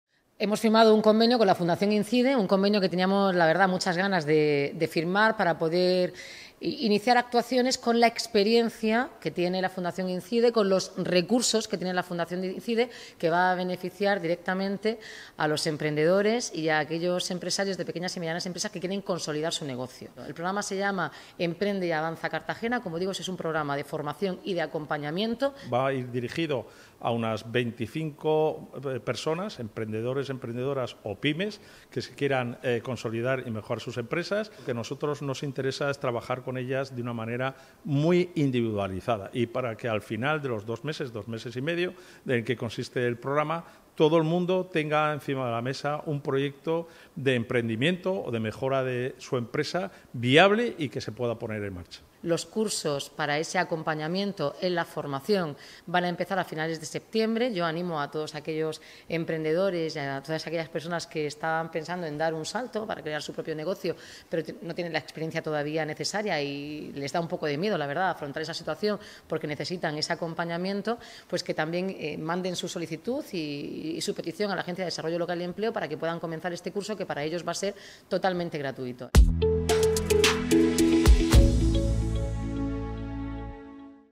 El Ayuntamiento y la Fundación INCYDE colaborarán en la formación de emprendedores de Cartagena
El Ayuntamiento de Cartagena y la Fundación Instituto Cameral para la Creación y Desarrollo de la Empresa (INCYDE), han acordado participar en una experiencia formativa dirigida al emprendimiento y a la promoción del autoempleo. El programa “Emprende y Avanza Cartagena”, cofinanciado por el Fondo Social Europeo, se desarrollará con el objetivo de impulsar la creación y consolidación de nuevas empresas en el municipio.

Este innovador programa formativo será gestionado por INCYDE a través de la Agencia de Desarrollo Local y Empleo (ADLE) por lo que según la alcaldesa, “va a permitir que se fortalezcan los recursos de ADLE para promover el crecimiento profesional de personas desempleadas, trabajadoras, autónomas y empresarias de Cartagena”.

Los participantes, que recibirán una formación integral en conocimientos y habilidades empresariales, podrán beneficiarse de una serie de actividades diseñadas para fortalecer sus capacidades emprendedoras, incluyendo talleres, sesiones de mentoría y seguimiento continuo. La alcaldesa ha recordado que “los cursos empezarán a finales de septiembre” y también ha animado a “aquellas personas que estén pensando en emprender a que manden su solicitud a la ADLE”.

Con una subvención directa de 17,371.20 euros por parte de ADLE y una contribución de 26,056.80 euros de INCYDE, el programa se desarrollará con la finalidad de promover el éxito empresarial y generar nuevas oportunidades de crecimiento en la región. En este sentido, Noelia Arroyo ha asegurado que “esta relación con INCYDE va a dar muy buenos resultados para el municipio ya que la fundación tiene una gran experiencia también en materia turismo de calidad, digitalización y comercio”.

Los interesados en participar en “Emprende y Avanza Cartagena” tendrán acceso gratuito a una formación de calidad, respaldada por expertos en el campo del emprendimiento, que les permitirá desarrollar sus proyectos empresariales y contribuir al desarrollo económico local.

Este programa de formación es uno de los acuerdos derivados de la nueva etapa de colaboración entre ambas instituciones en las que se ha trabajado desde la reunión celebrada en marzo pasado entre la alcaldesa y el director general de la Fundación INCYDE Javier Collado, en la que se analizó el papel de la fundación en la estrategia formativa puesta en marcha por el ayuntamiento de Cartagena para fomentar el empleo e impulsar la creación de empresas en Cartagena.